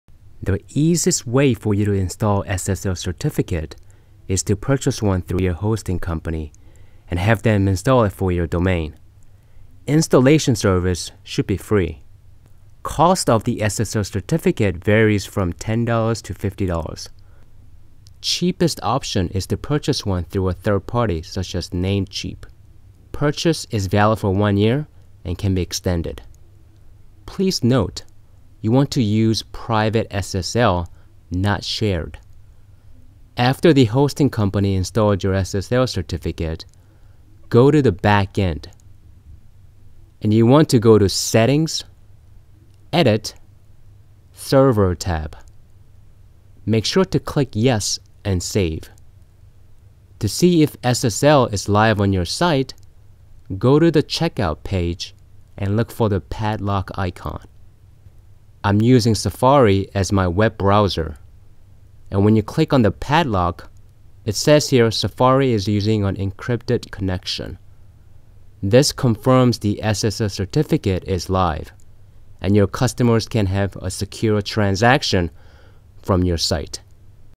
How to Install SSL Certificate in OpenCart 2.0 - 2.X Video Tutorials for Beginners 20
Get SSL Certificate for $9.00 a year through Namecheap

The easiest way for you to install SSL certificate is to purchase one through your hosting company and have them install it for your domain. Installation service should be free.  Cost of the SSL certificate varies from $10 to $50. Cheapest option is to purchase one through a third party such as Namecheap. Purchase is valid for one year and can be extended. Please note, you want to use Private SSL not Shared. After the hosting company installed your SSL certificate, go to the backend. And you want to go to Settings - Edit - Server tab. Make sure to click Yes and Save. To see if SSL is live on your site, go to the checkout page and look for the padlock icon. I’m using Safari as my browser and when you click on the padlock, it says here Safari is using an encrypted connection. This confirms the SSL certificate is live and your customers can have a secure transactions from your site.